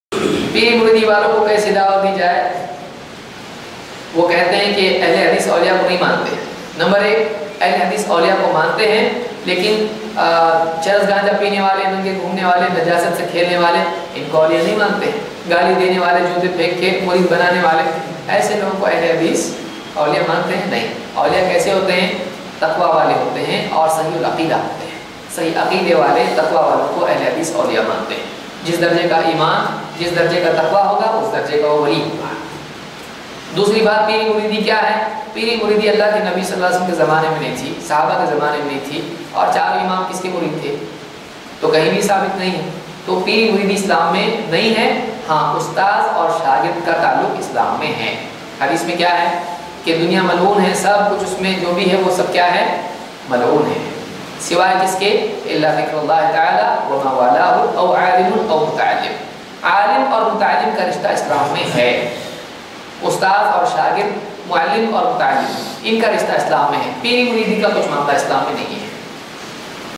Peeri mooridi walo ko kaise dawat de | Abu Zaid Zameer
This Questions and Answer is taken from :
Abu Zaid Zameer Tour at Makami Jamiat Ahle Hadees, Aakot
Maharashtra.
Date: 5  and 6 Feb 2013